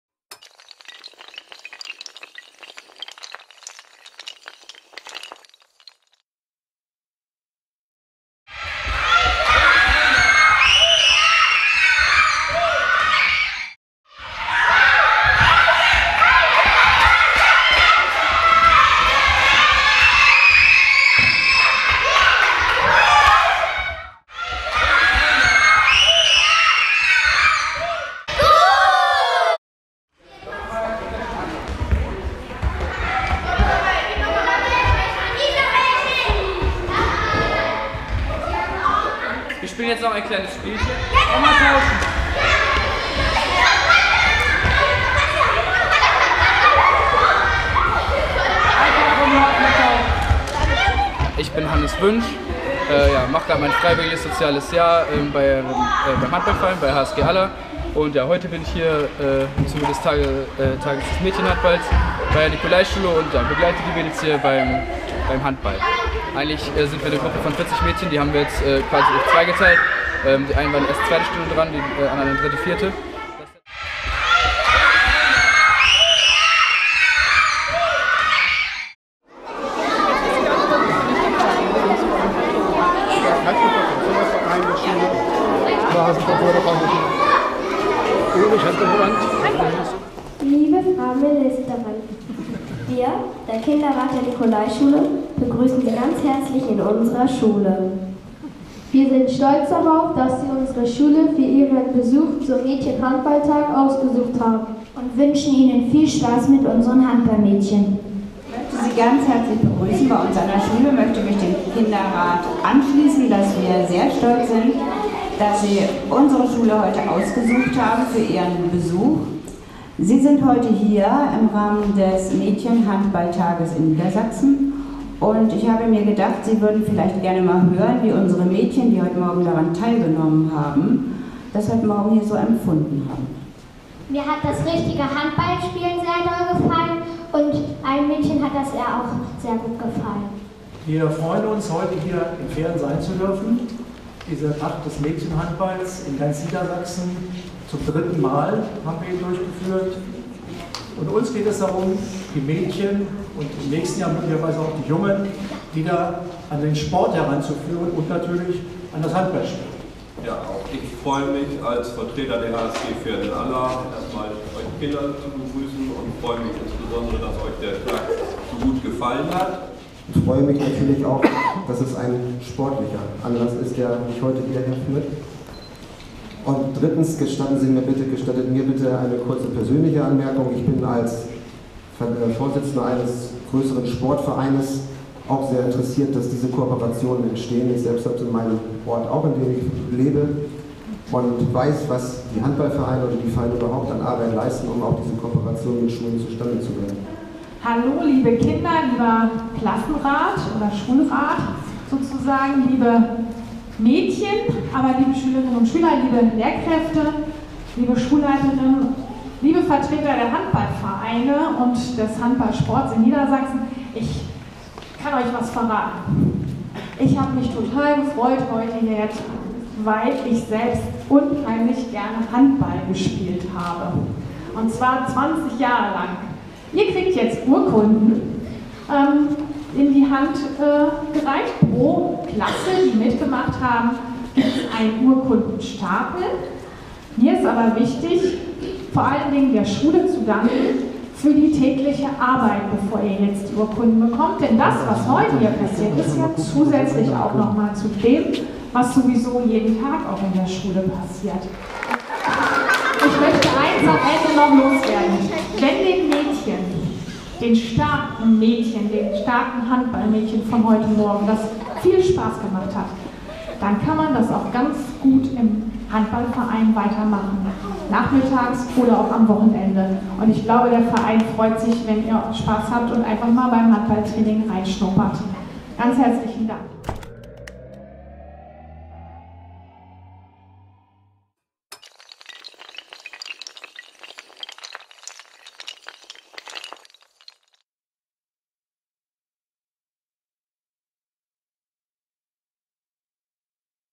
Tag des Mädchenhandballs 2014
12 390 Mädchen aus 174 niedersächsischen Grundschulen beteiligten sich am Freitagam „Tag des Mädchenhandballs“. Schirmherrin des Aktionstages war Niedersachsens Kultusministerin Frauke Heiligenstadt. Sie besuchte die Nicolaischule in Verden. Gefördert wurde die Aktion unter anderem vom Freundeskreis des Deutschen Handballs.